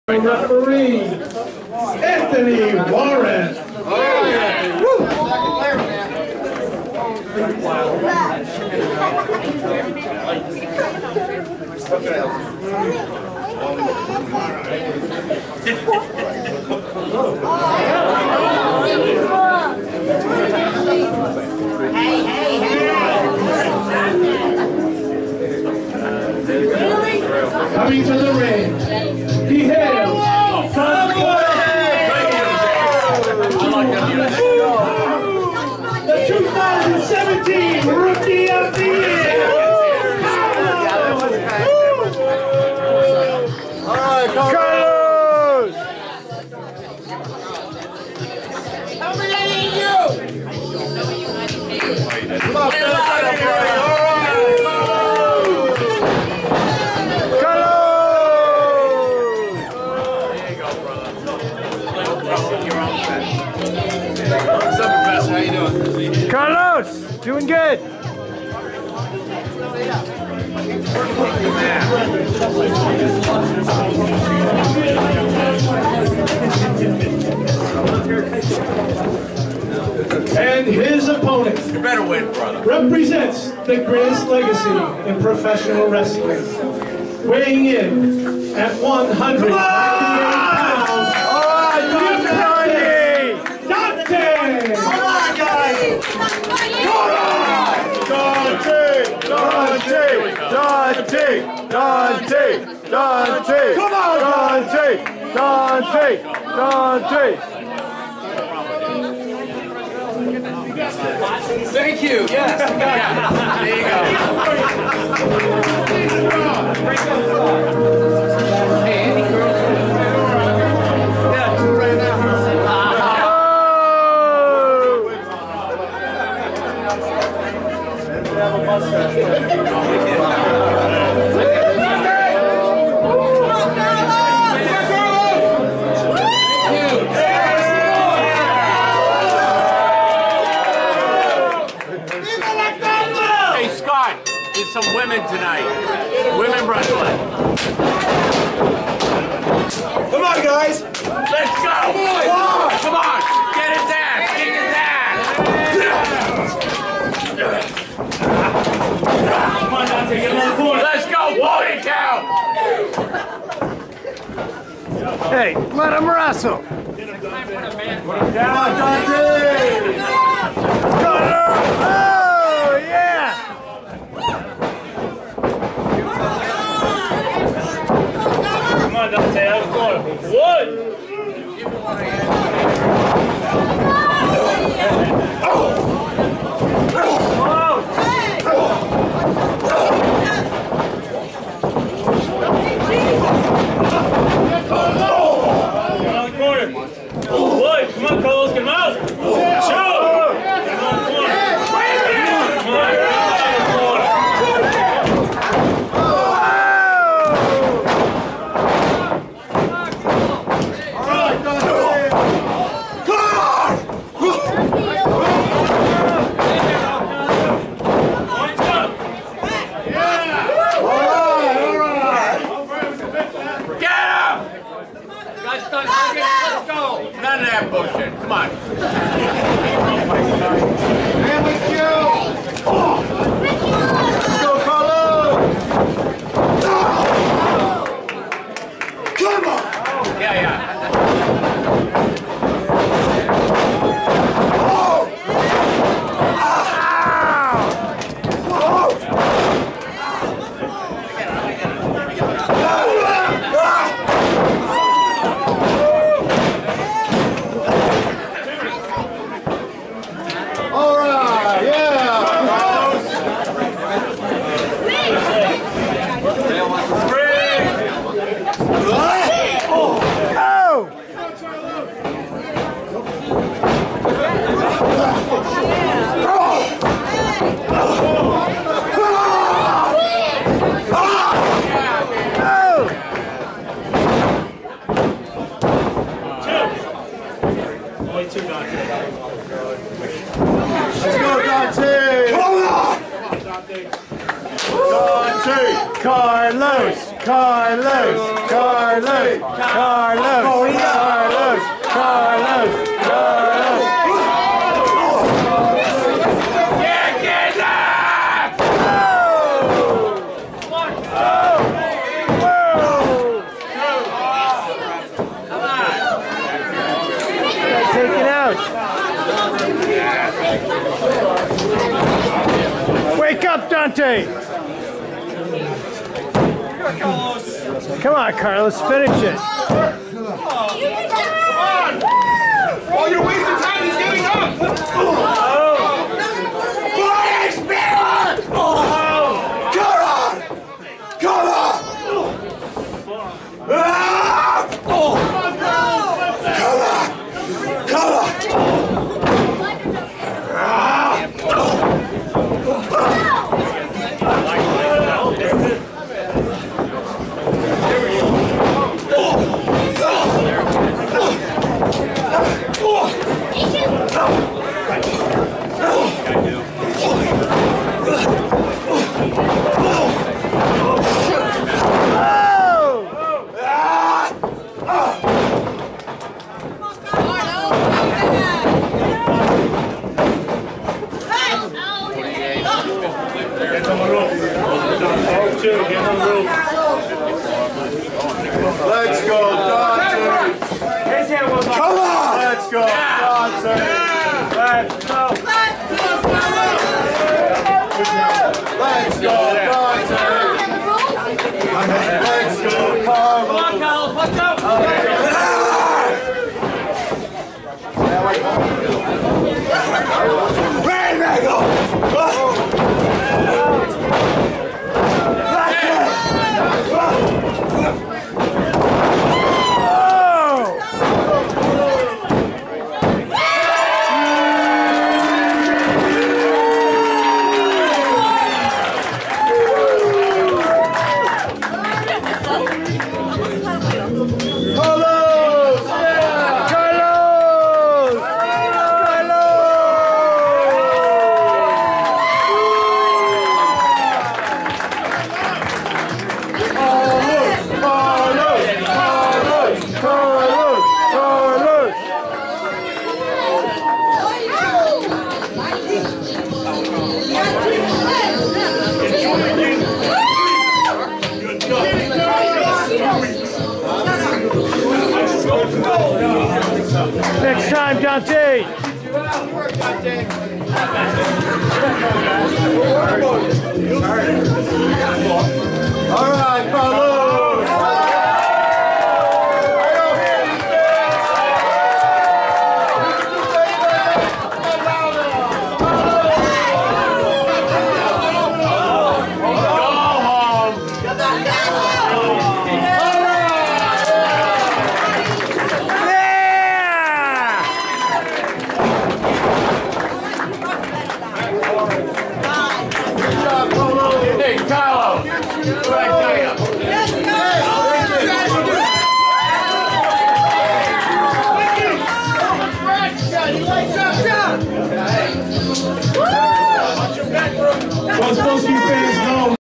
Carlos v. Dante SPW 4 Feb. 2017
Carlos faces The Apprentice Dante at Showcase Pro Wrestling in Attleboro, MA USA 4 Feb. 2017. wrestlingprofessor.wordpress.com